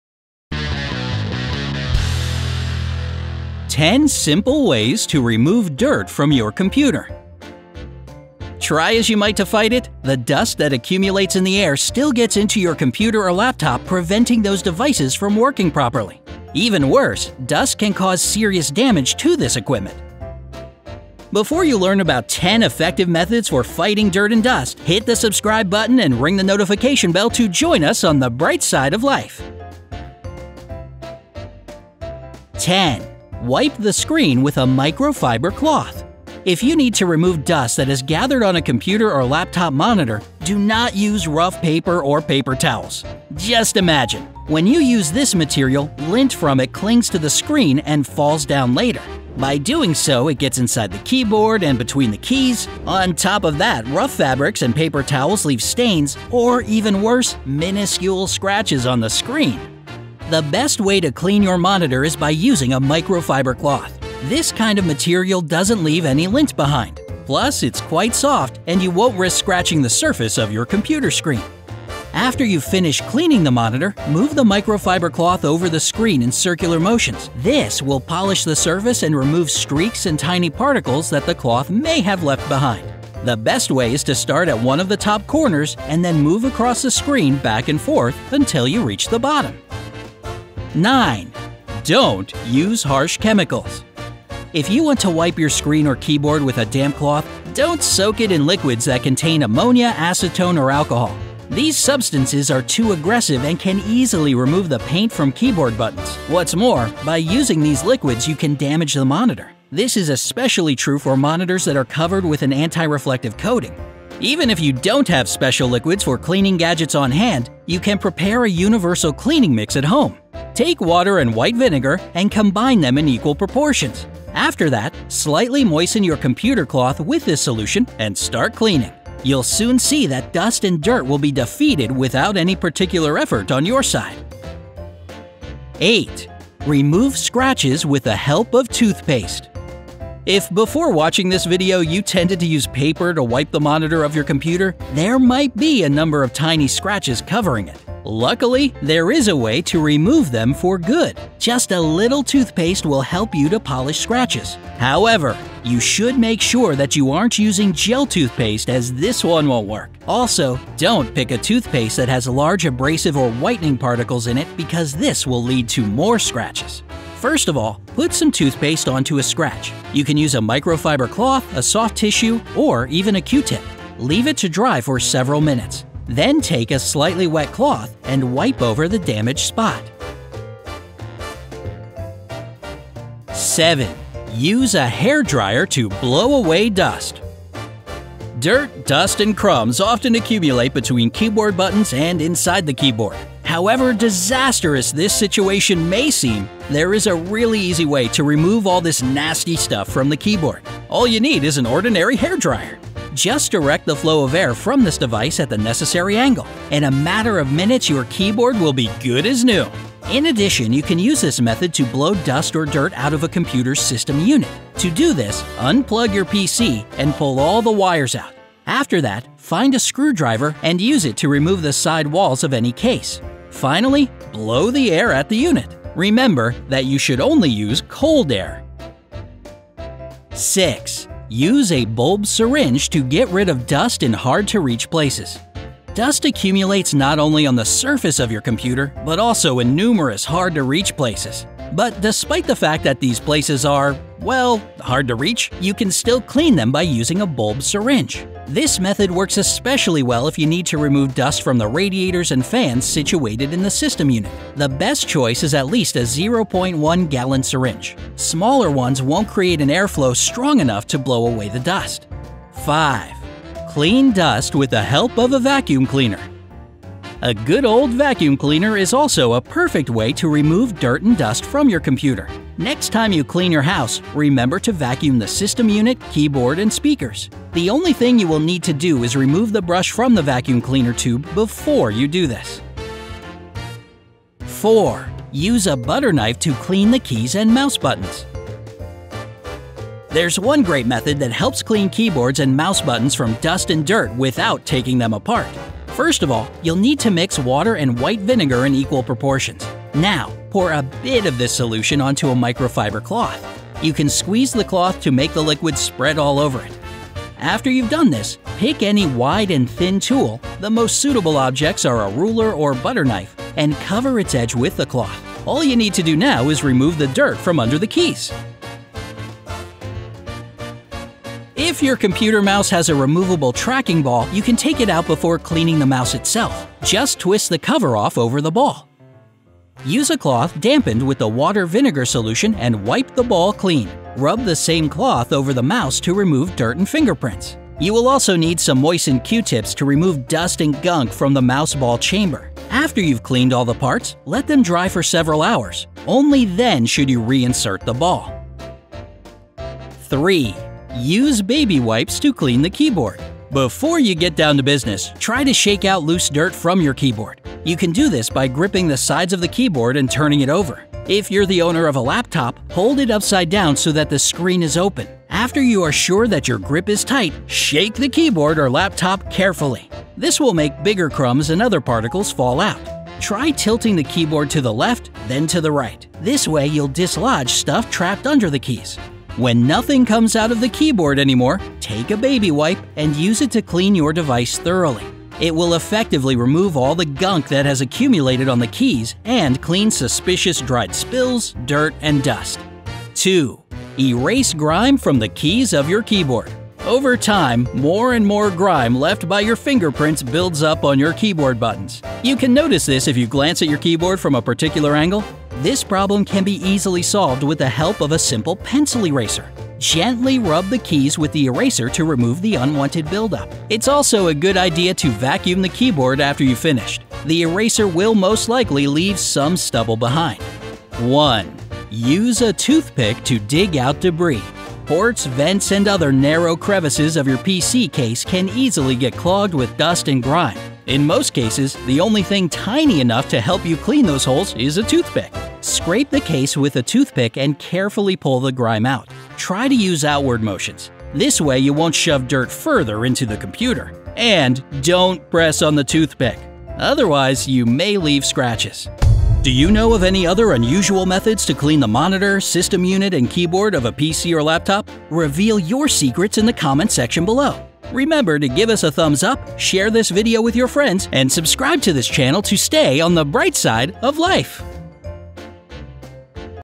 10 Simple Ways to Remove Dirt From Your Computer
Try as you might to fight it, the dust that accumulates in the air still gets into your computer or laptop, preventing these devices from working properly. Even worse, dust can cause serious damage to this equipment. Actually, dust is one of the main enemies of your computer. Bright Side collected some tips on how to easily clean your computer from dust and dirt with the help of available items.

Did you know that a good old vacuum cleaner is also a perfect way to remove dirt and dust from your computer? Next time you clean your house, remember to vacuum the system unit, keyboard, and speakers. The only thing you will need to do is remove the brush from the vacuum cleaner tube before you do this. Do you wanna know other advice? Then watch the video!

TIMESTAMPS:
Wipe the screen with a microfiber cloth 0:34
Don't use harsh chemicals 1:31
Remove scratches with the help of toothpaste 2:16
Use a hair dryer to blow away dust 3:04
Use a bulb syringe to get rid of dust in hard-to-reach places 3:51
Clean dust with the help of a vacuum cleaner 4:24
Use a butter knife to clean the keys and mouse buttons 4:44
Use baby wipes to clean the keyboard 5:52
Erase grime from the keys of your keyboard 6:33
Use a toothpick to dig out debris 7:02

SUMMARY:
- If you need to remove dust that has gathered on a computer or laptop monitor, do not use rough paper or paper towels. When you use this material, lint from it clings to the screen and falls down later. The best way to clean your monitor is by using a microfiber cloth.
- If you want to wipe your screen or keyboard with a damp cloth, don't soak it in liquids that contain ammonia, acetone, or alcohol. These substances are too aggressive and can easily remove the paint from keyboard buttons.
- Just a little toothpaste will help you to polish scratches. However, you should make sure that you aren't using gel paste as this one won't work. Also, don't pick a toothpaste that has large abrasive or whitening particles in it because this will lead to more scratches.
- Dirt, dust, and crumbs often accumulate between keyboard buttons and inside the keyboard. All you need is an ordinary hair dryer!
- Dust accumulates not only on the surface of your computer but also in numerous hard-to-reach places. But despite the fact that these places are, well, hard to reach, you can still clean them by using a bulb syringe.
- If your computer mouse has a removable tracking ball, you can take it out before cleaning the mouse itself. Just twist off the cover over the ball. Use a cloth dampened with the water-vinegar solution to wipe the ball clean.
- Before you get down to business, try to shake out loose dirt from your keyboard. You can do this by gripping the sides of the keyboard and turning it over. If you're the owner of a laptop, hold it upside down so that the screen is open.
- Over time, more and more grime left by your fingertips builds up on your keyboard buttons. This problem can be easily solved with the help of a simple pencil eraser.
- Ports, vents, and other narrow crevices of your PC case can easily get clogged with dust and grime. In most cases, the only thing tiny enough to help you clean these holes is a toothpick!